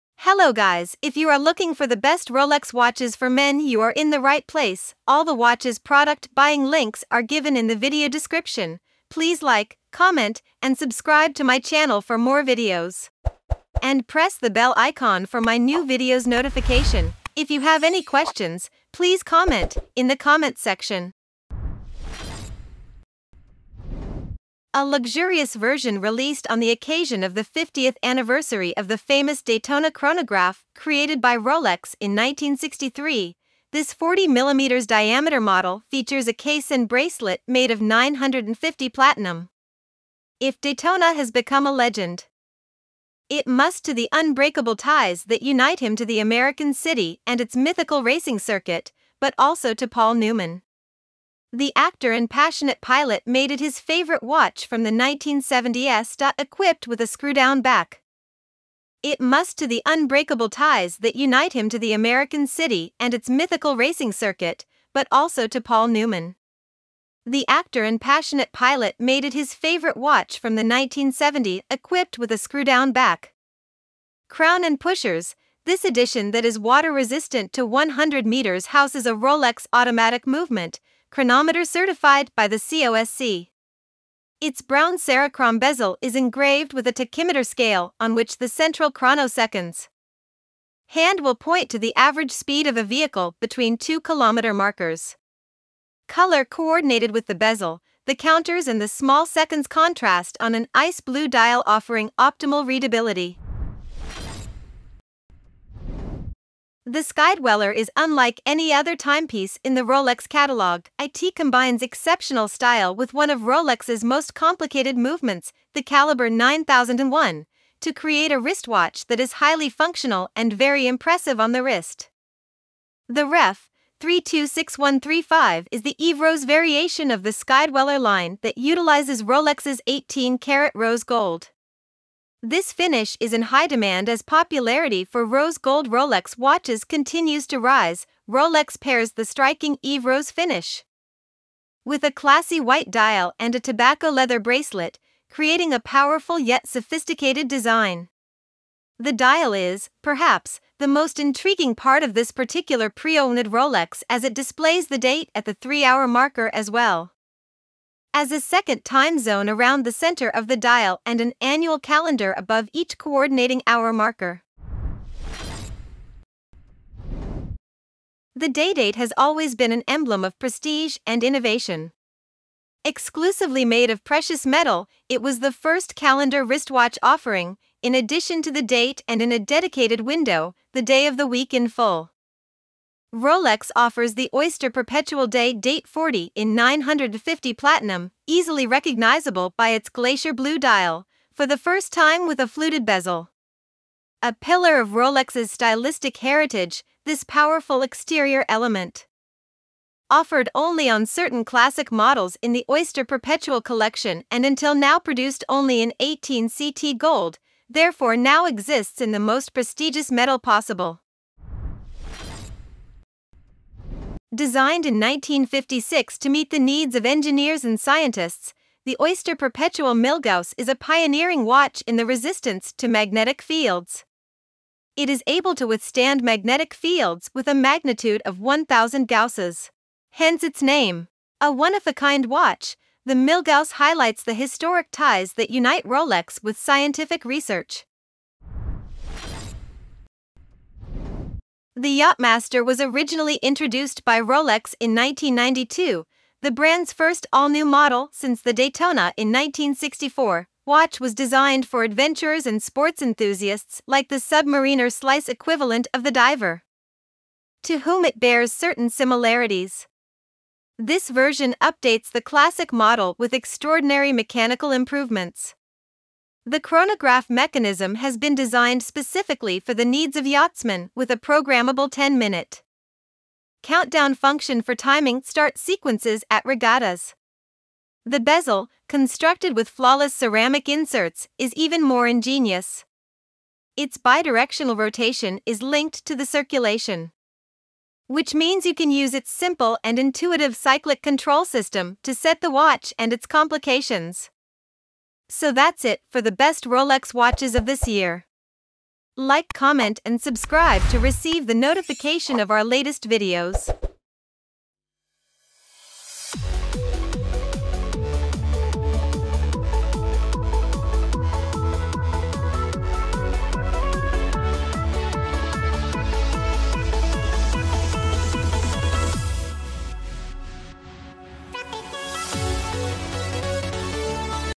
Top 5 Best Rolex Watches To Buy in 2022
⌚ 1. Rolex Cosmograph Daytona
⌚ Amazon International
⌚ 2. Rolex  Sky-Dweller
⌚ Amazon International
⌚ 3.Rolex Day-Date 40
⌚ Amazon International
⌚ 4. Rolex Milgauss
⌚ Amazon International
⌚ 5. Rolex Yacht-Master II
⌚ Amazon International